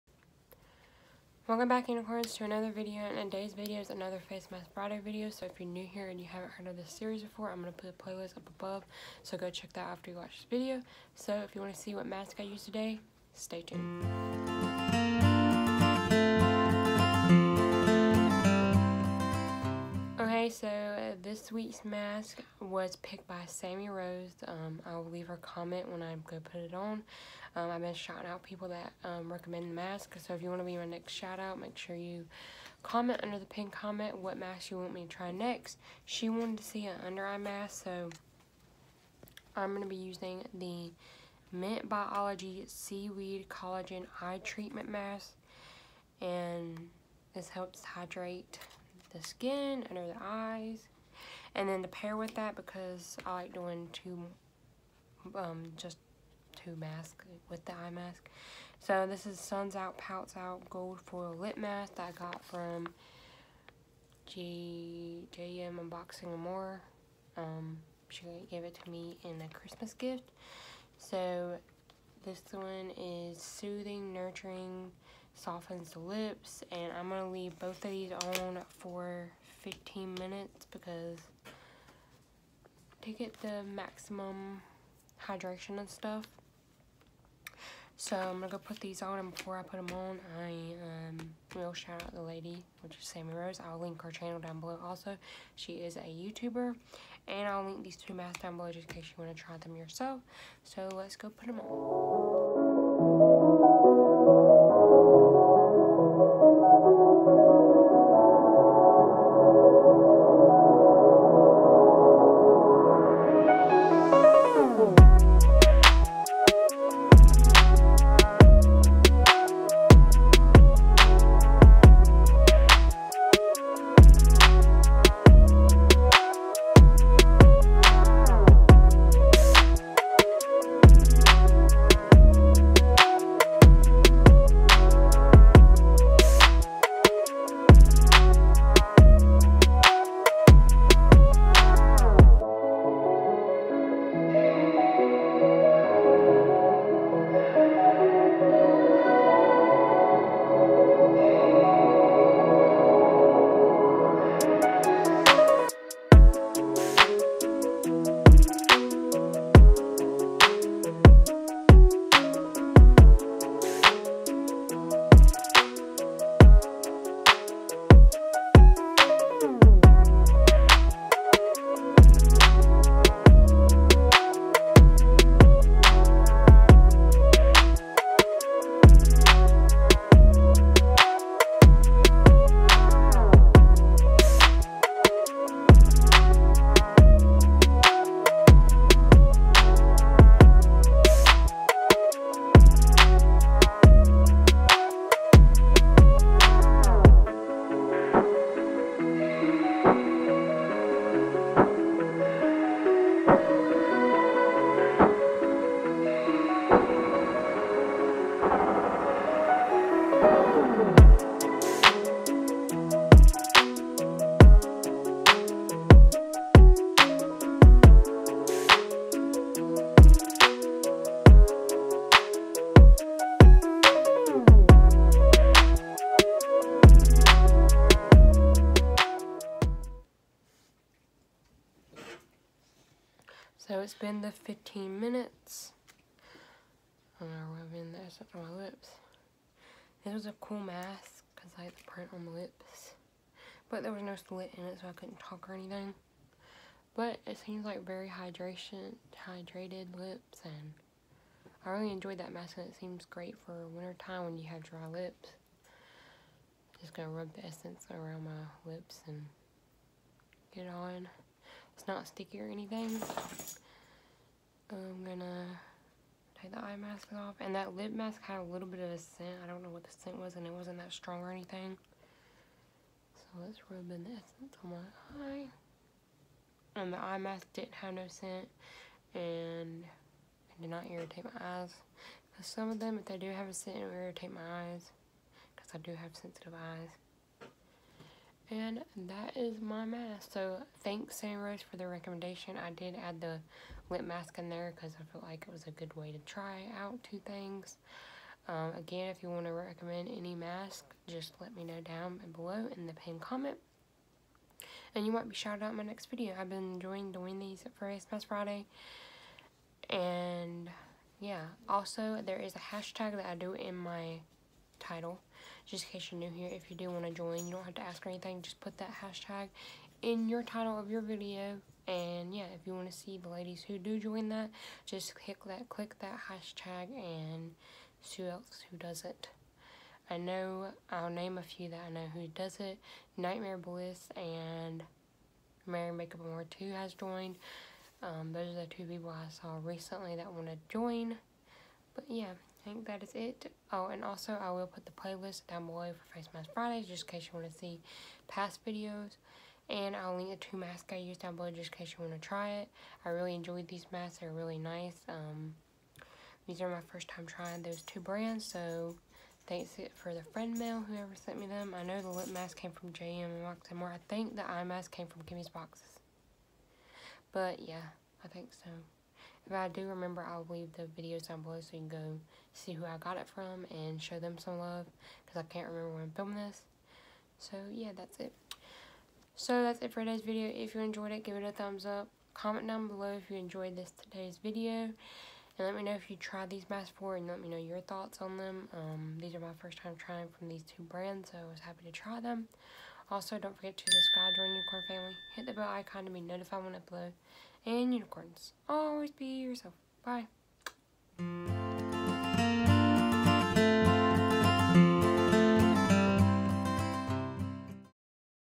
Gold Lip & Undereye Masks
Welcome unicorns to another video. In today's video I am going to be trying these gold under eye and lip masks for this week's facemask Friday.

Lip mask: